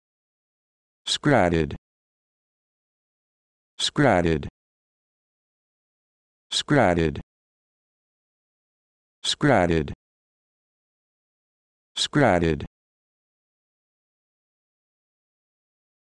How to Pronounce Scratted
This video shows you how to pronounce Scratted